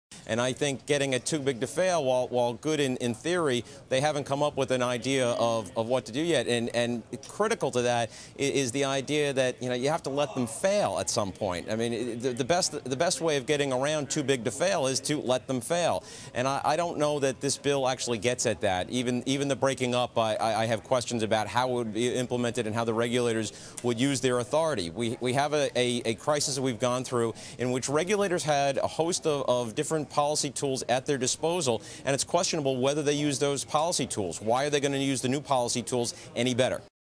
Keefe Bruyette's Gardner Discusses Financial Oversight: Video
Nov. 10 (Bloomberg) -- Brian Gardner, an analyst at Keefe Bruyette & Woods Inc., talks with Bloomberg Television about Senate Banking Committee Chairman Chris Dodd's proposed legislation to overhaul the U.S. financial regulatory system. (This report is an excerpt. Source: Bloomberg)